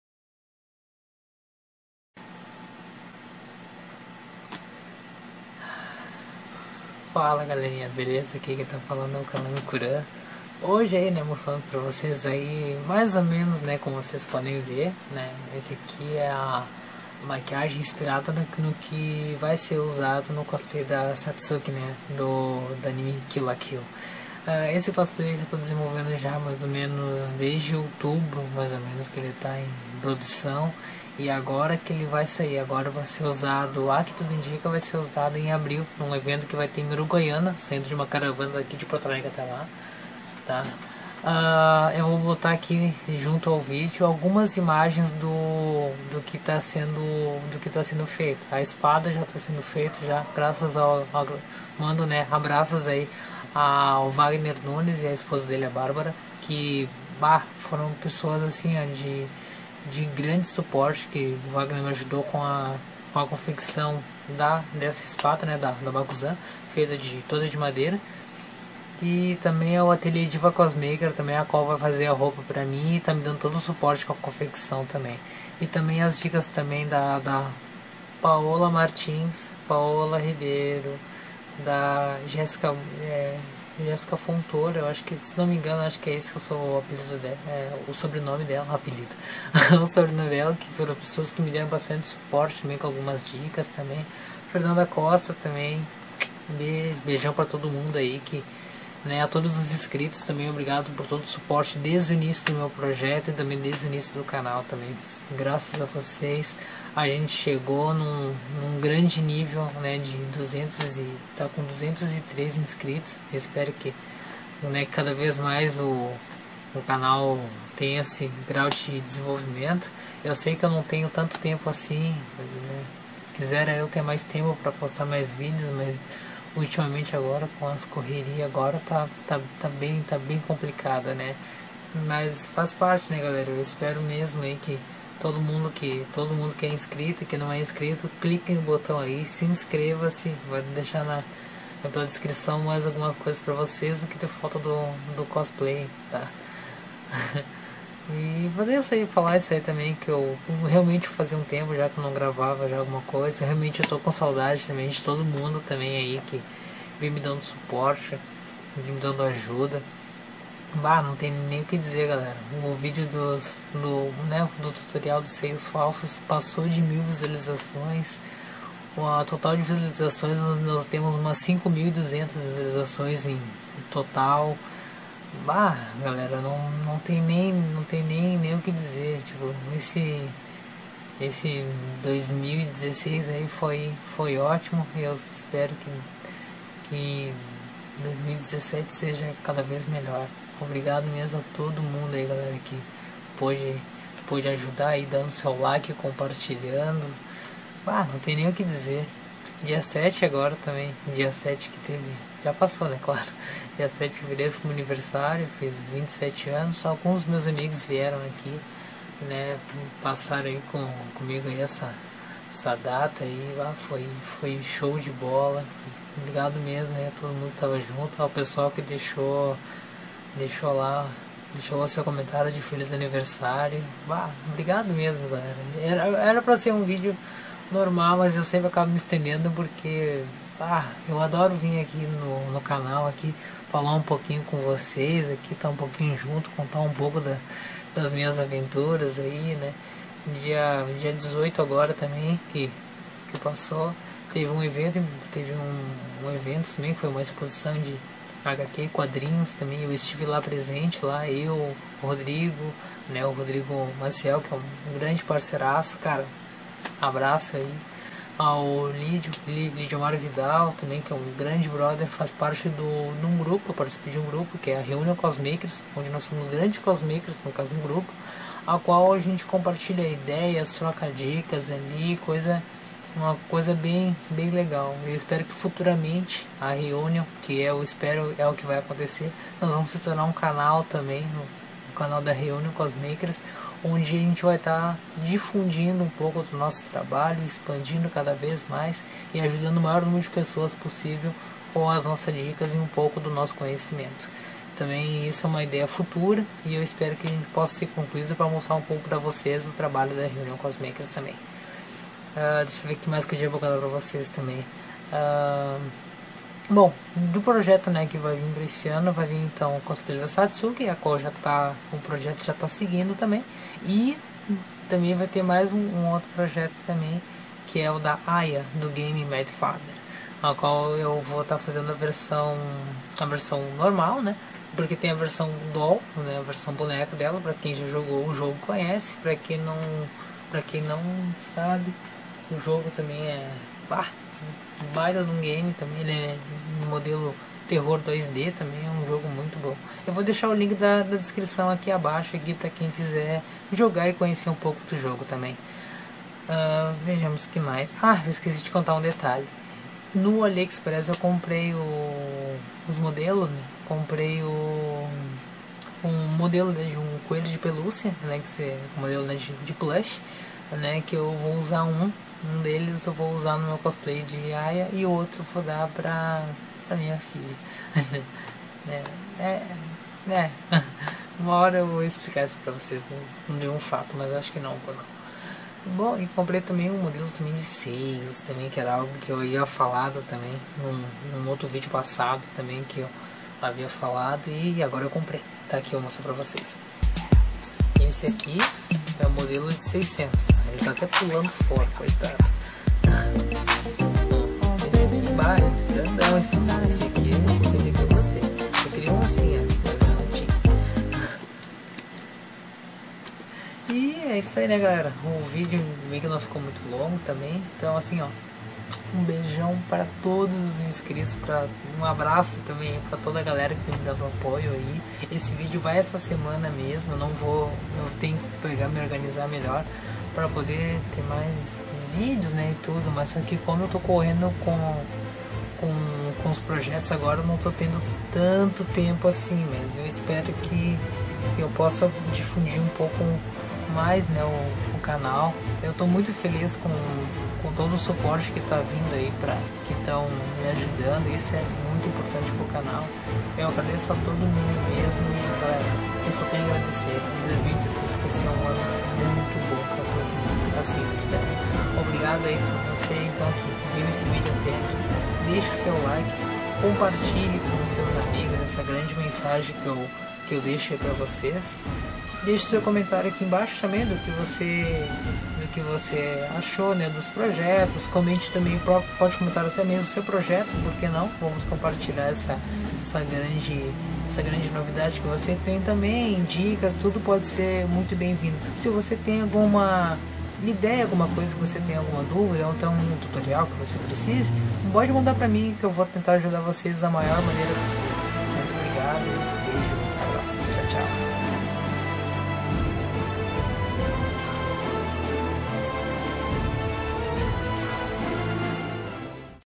Satsuki Project : Novidades e mais projetos
fala ae galerinha , falando nesse um pouco dos projetos para esse ano, as melhorias e os views do canal , falarei um pouco das compras que fiz no aliexpress, as grandes coisas que me aconteceram de bom , e melhores virão. teve até um momento de descontração kkk (bem zoeiro mesmo ). gostaria de agradecer a todos que viram esse vídeo , deixem seu comentário , deixem seu like , compartilhem com seus amigos .  abraços !!